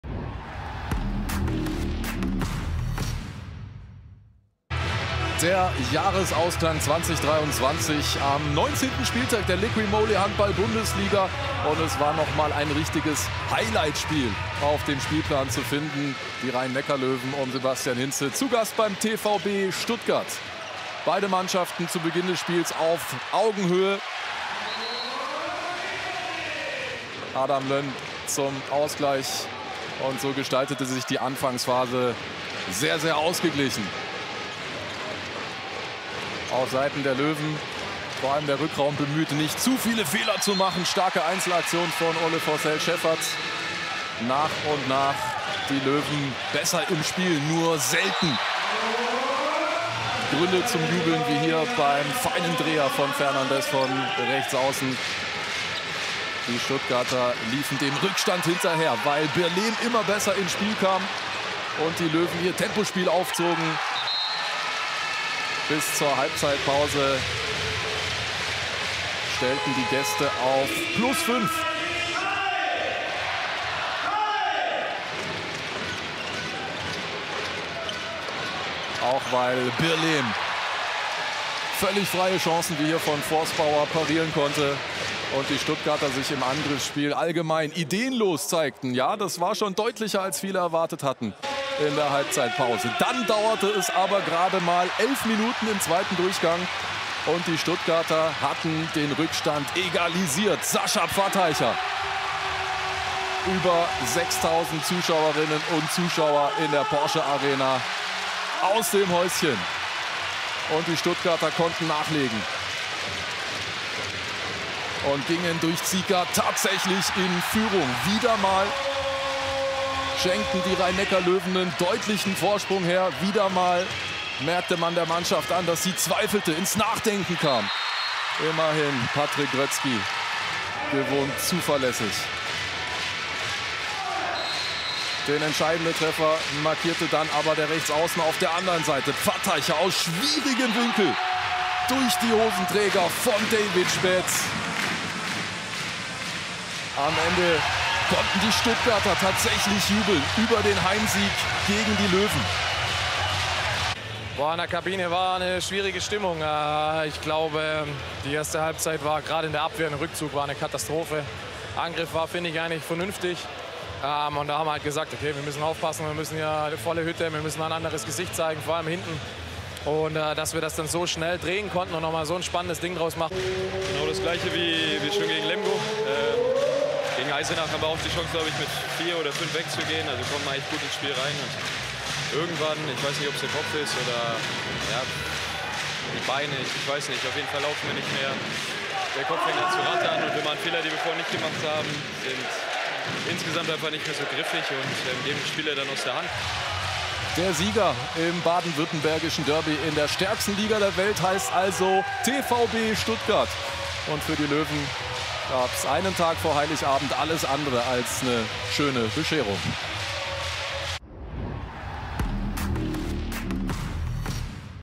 TVB Stuttgart vs. Rhein-Neckar Löwen | Spiel Highlights
Am gestrigen Samstagabend konnten sich die WILD BOYS im Derby mit gegen die Rhein-Neckar Löwen durchsetzen. 🤩
Die Highlights der Partie findet ihr in diesem Video.

Alle Spiele der LIQUI MOLY HBL live und auf Abruf bei Dyn